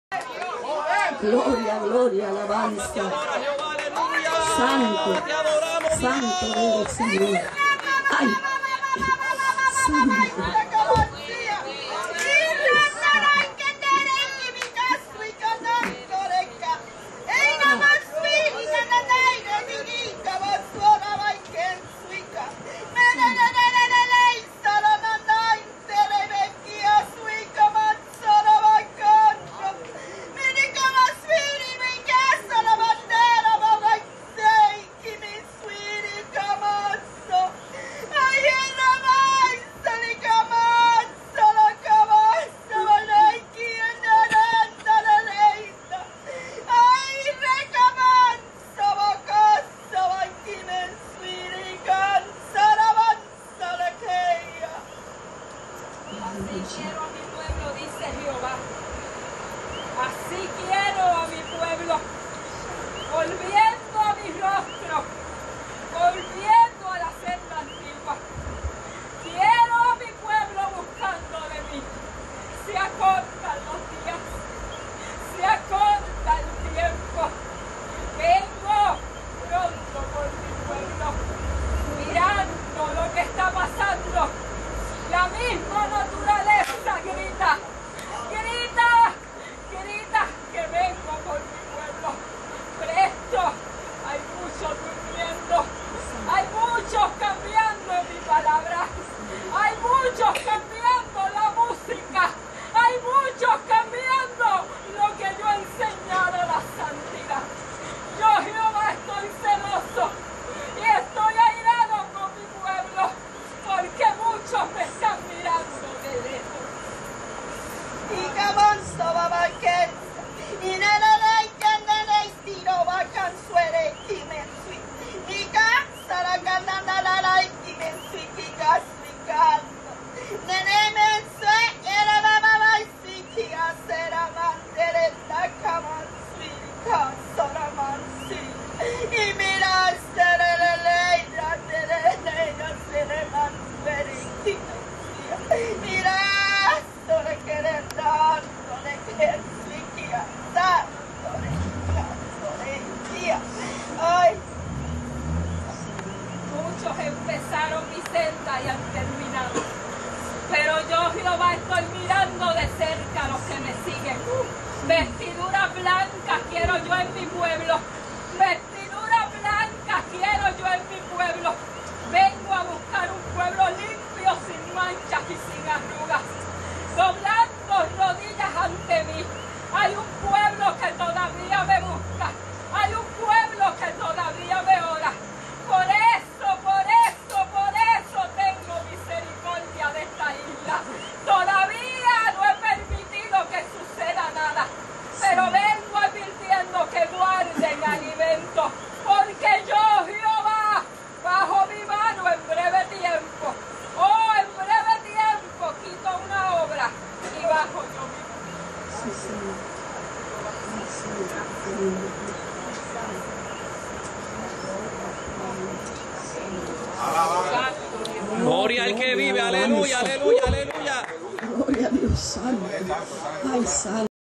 Primera Iglesia de Dios Fajardo "Profecia" 3/1/10
En un culto en honor a los de la edad de oro Dios le habla su pueblo otra vez:"Volveos a la senda antigua". Dios esta airado de muchas iglesia por haber dejado el primer amor y las primeras obras. "Volveos a mi y YO me volvere a vosotros. dice Jehova" Aun teniendo misericordia por la isla del cordero por las oraciones de aquellos que aun no han doblado rodillas ante los baales.